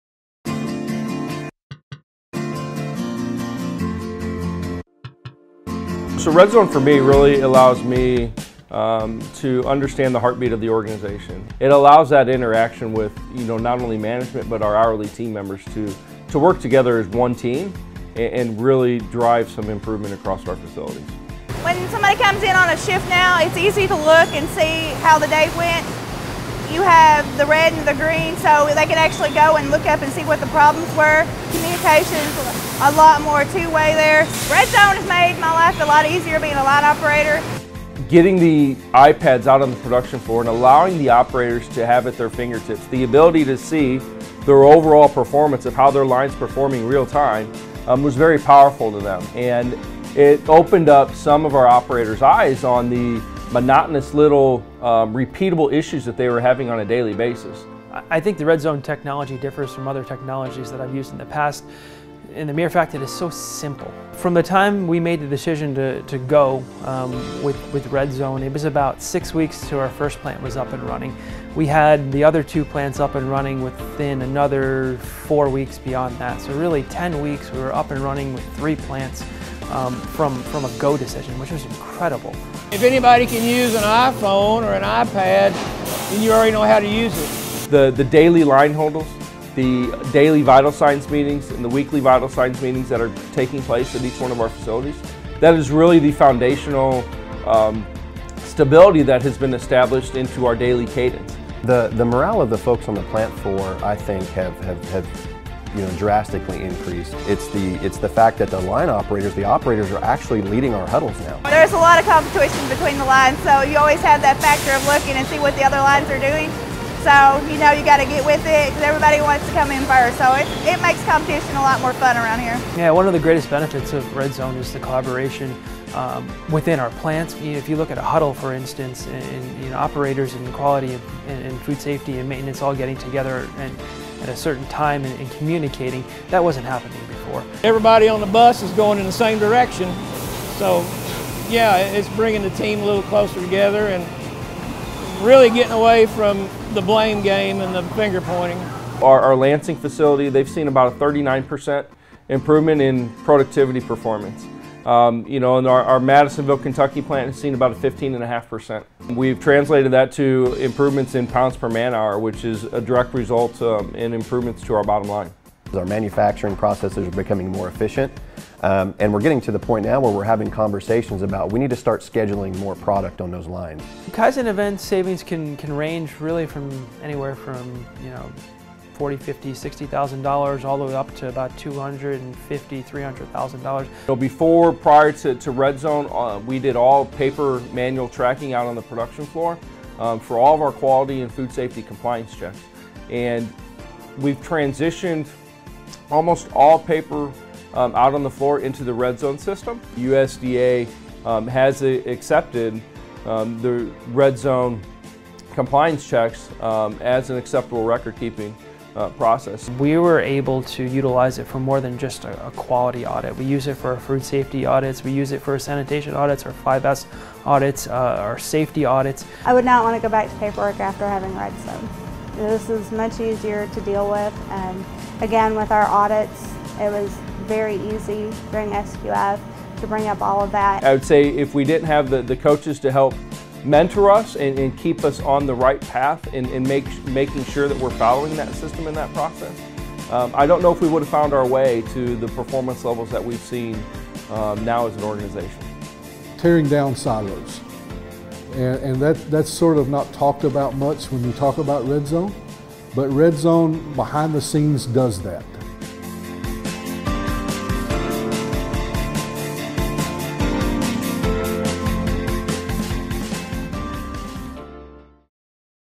Land O'Frost and the Redzone Production System
See how premium sliced meat producers, Land Oâ€™Frost, increased their productivity by 39% with the Redzone Production System.